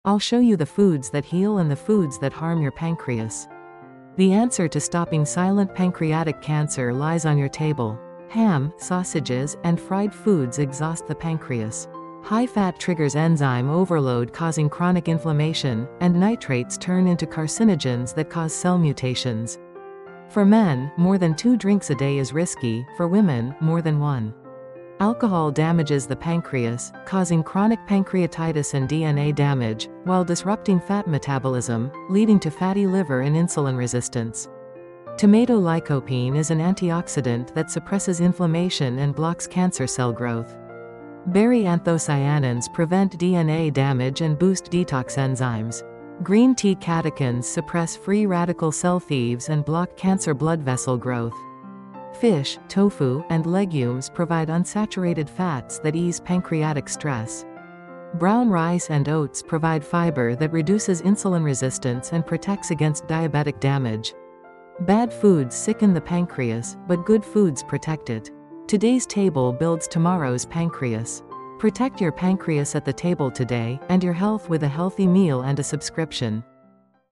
FOODS THAT HEAL OR HARM YOUR PANCREAS
Pancreatic cancer is called the “silent killer.”
What you eat every day can either damage your pancreas or protect it.

Processed meats like ham and sausages, fried foods, and excess alcohol overload the pancreas, trigger chronic inflammation, DNA damage, fatty liver, and insulin resistance—major risk factors for pancreatic cancer.

But there is hope.
Tomatoes (lycopene), berries (anthocyanins), and green tea (catechins) provide antioxidants that block inflammation, DNA damage, and cancer cell growth.
Fish, tofu, and legumes supply healthy unsaturated fats, while brown rice and oats reduce insulin resistance and protect against diabetic damage.

Bad foods can destroy your pancreas, but good foods can save it.
Your choice at the table today could shape your health tomorrow.